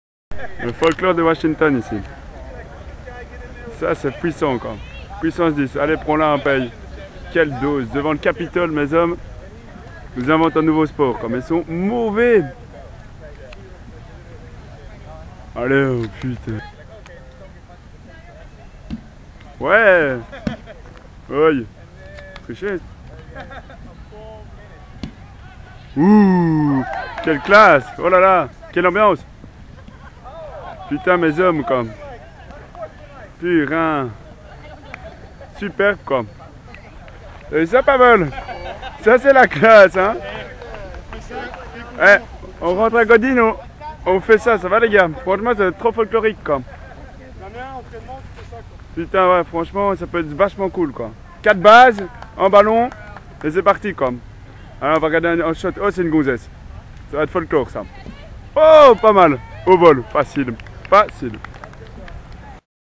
Nouveau sport
A washington, devant le capitole, ils jouent a je ne sais pas quoi lol